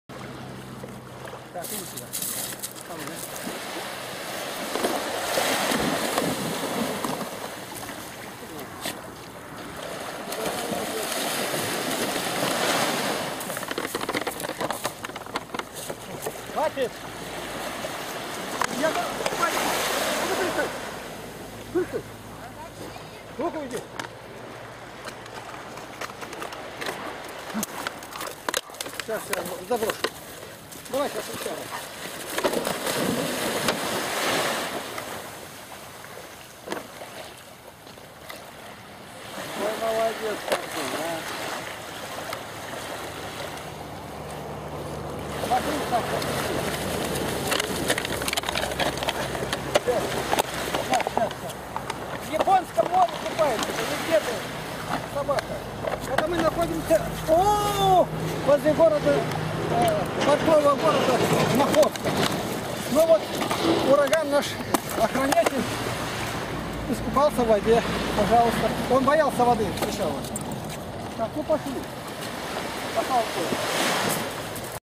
Ураган и Японское море. 7. 2012 Игнатий Лапкин
Вопрос 920: 3 т. Есть такой рассказ: «Недостойный священник крестил мальчика. На следующий день бабушка говорит внуку, проходя по церкви: «Поздоровайся со священником, который тебя крестил вчера». А мальчик ответил: «Нет, этот дедушка вчера лежал связанный под лавкой, вон в том углу, а меня крестил другой, молодой, весь в светлом, в блестящей одежде. Сегодня я его не видел». Так что же это получается, что наши священники может нас совсем и не крестили?

Ответ: Этот рассказ не из Евангелия, но он встречается и в житиях святых, что за недостойного пастыря его функции на себя берет Ангел. И даже выполняют роль священника нечистые духи (см. Житие Макария Александрийского, 19 января, и Андрея Христа ради юродивого, 4 октября). Да даст нам Господь истинных пастырей, имеющих страх Божий и творящих дело своё точно по канонам, установленным отцами вселенской церкви.